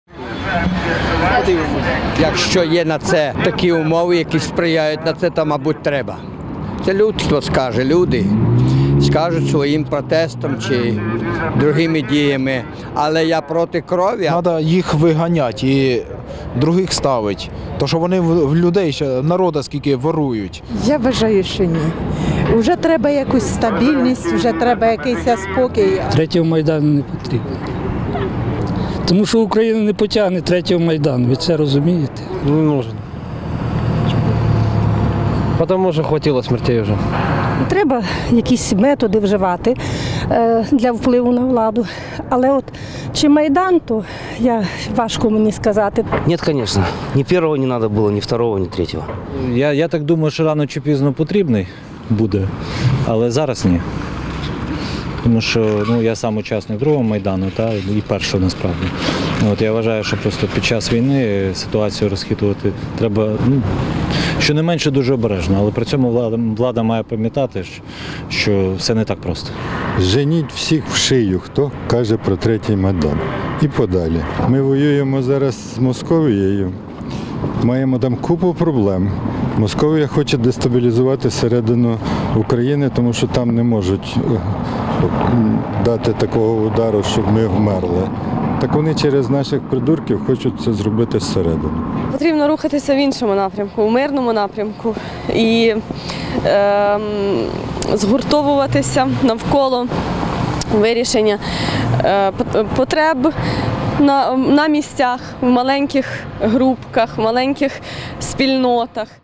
Чи потрібен Україні третій Майдан?
До третьої річниці перемоги Майдану та дня Героїв Небесної сотні у Києві відбудеться низка масових акцій. Від цих вихідних і до 22 лютого різні праворадикальні організації проводитимуть мітинги. І хоч назви організацій різні, але гасла схож: невдоволення владою, заклики до перевиборів парламенту і президента, боротьба з олігархами. Деякі – закликають до третього Майдану. Радіо Свобода запитало перехожих у центрі Києва: чи потрібна зараз Україні революція?